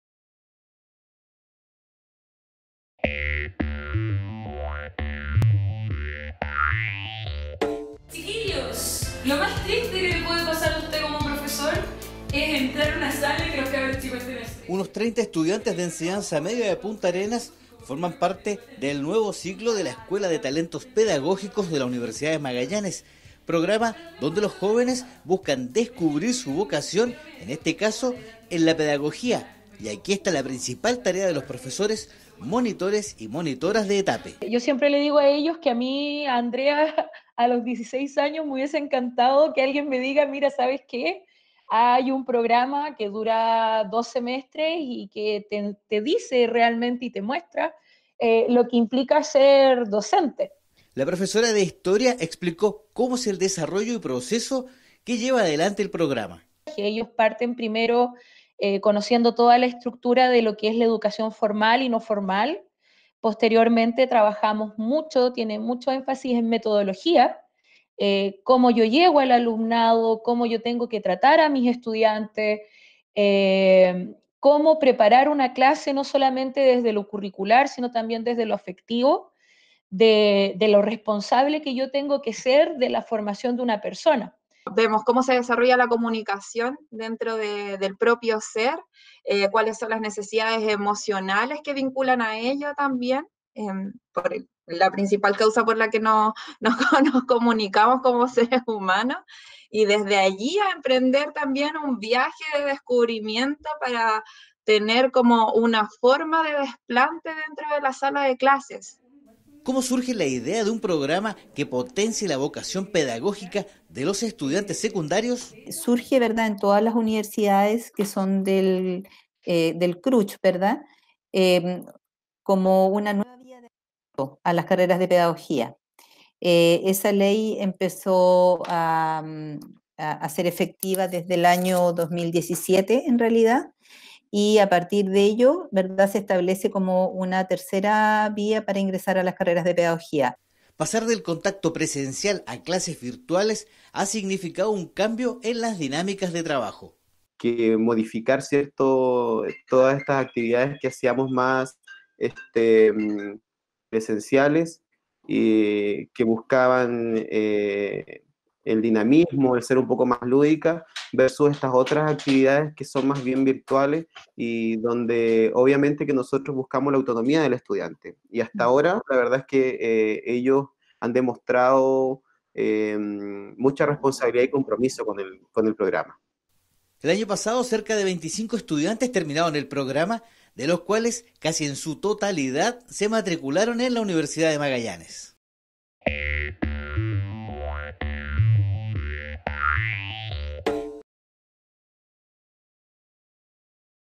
30 estudiantes comenzaron a cursar en la Escuela de Talentos Pedagógicos de la UMAG
El viernes 8 de mayo se dio inicio a la Escuela de Talentos Pedagógicos (ETAPE), retomando las actividades con los estudiantes que comenzaron el año pasado cuando cursaban 3° medio.

Los alumnos desarrollan 3 módulos: competencias comunicativas, vocación e identidad regional y pensamiento lógico matemático. Lo cual se llevará a cabo todos los viernes a partir de las 15.00 horas por medio de una plataforma de pregrado virtual de la Universidad de Magallanes, lo cual está programado hasta el 24 de julio.

El ETAPE comenzó el 2017, siendo abierto a todos los establecimientos de la comuna de Punta Arenas y a partir del año 2019 en Puerto Natales. En ambas comunas se cuenta con un Coordinador y tres profesionales a cargo de los módulos.